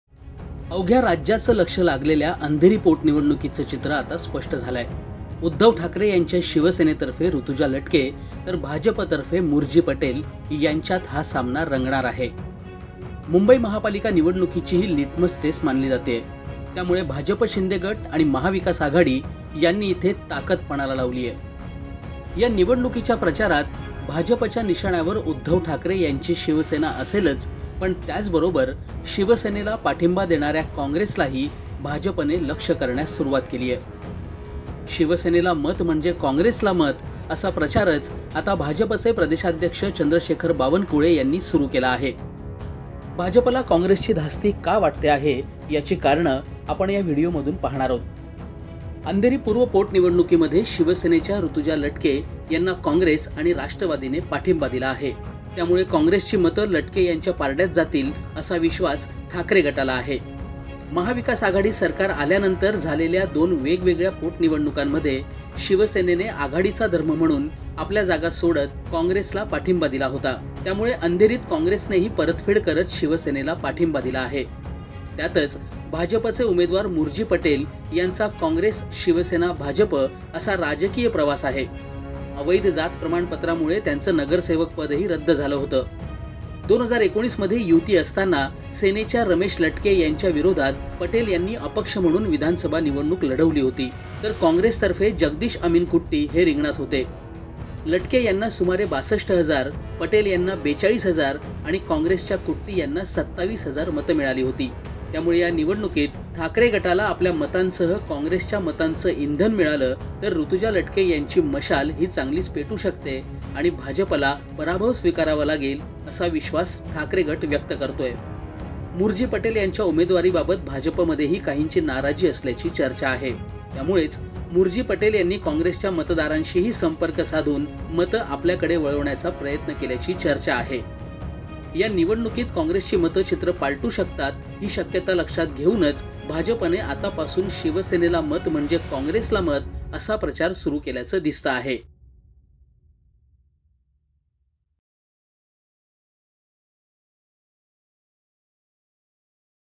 अंधेरी पूर्वमध्ये शिवसेनेसोबत काँग्रेस, भाजपचं टेन्शन वाढलं | Andheri Bypoll Election | Bhai Jagtap
अवघ्या राज्याचे लक्ष लागलेल्या अंधेरी पोटनिवडणुकीचे चित्र आता स्पष्ट झाले आहे. उद्धव ठाकरे यांच्या शिवसेनेतर्फे ऋतुजा लटके तर भाजपचे मुरजी पटेल यांच्यात हा सामना रंगणार आहे. मुंबई महापालिका निवडणुकीची ही लिटमस टेस्ट मानली जाते आहे. त्यामुळे भाजप-शिंदे गट आणि महाविकास आघाडी यांनी इथे ताकद पणाला लावली आहे. या निवडणुकीच्या प्रचारात भाजपच्या निशाण्यावर उद्धव ठाकरे यांची शिवसेना असेलच. पण त्याचबरोबर शिवसेनेला पाठिंबा देणाऱ्या काँग्रेसलाही भाजपने लक्ष्य करण्यास सुरूवात केली आहे. शिवसेनेला मत म्हणजे काँग्रेसला मत असा प्रचारच आता भाजपचे प्रदेशाध्यक्ष चंद्रशेखर बावनकुळे यांनी सुरू केला आहे. ( Sachin Joshi VO )